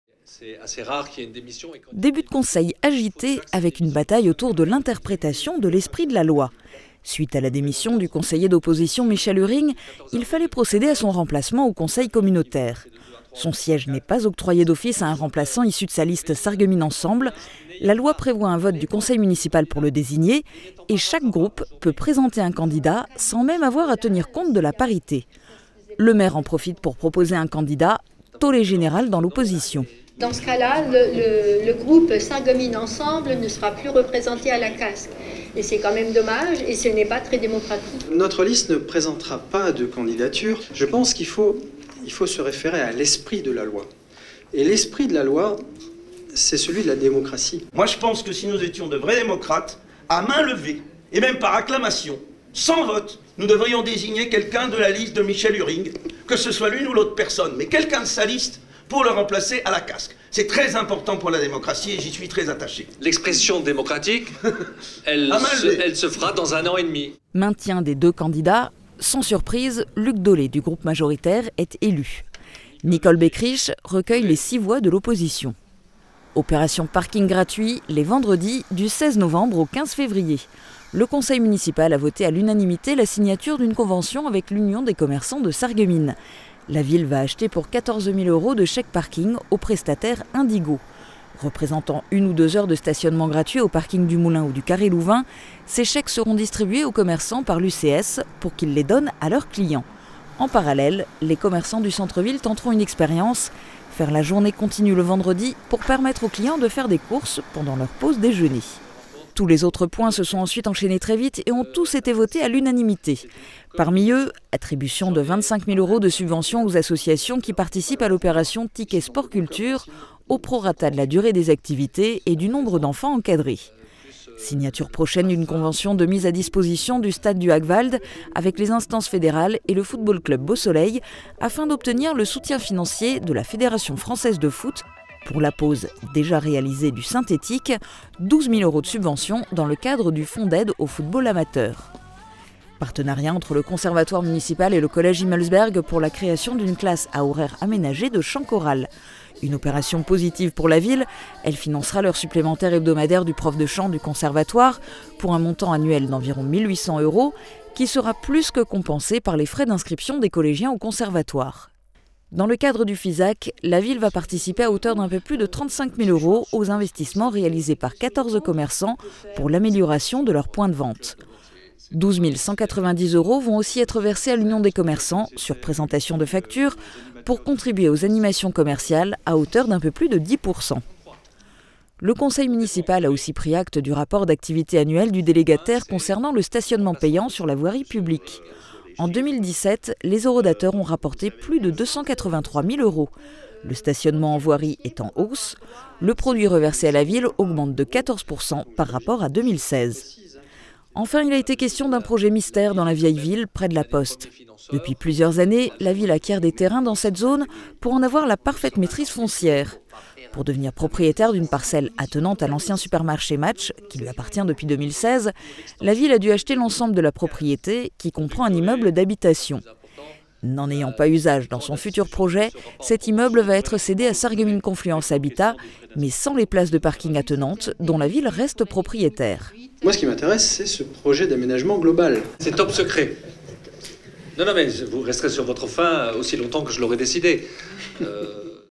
Conseil municipal de Sarreguemines
Élection et subventions au menu des discussions. Voici la synthèse expresse de quasiment 3 heures de conseil municipal à Sarreguemines.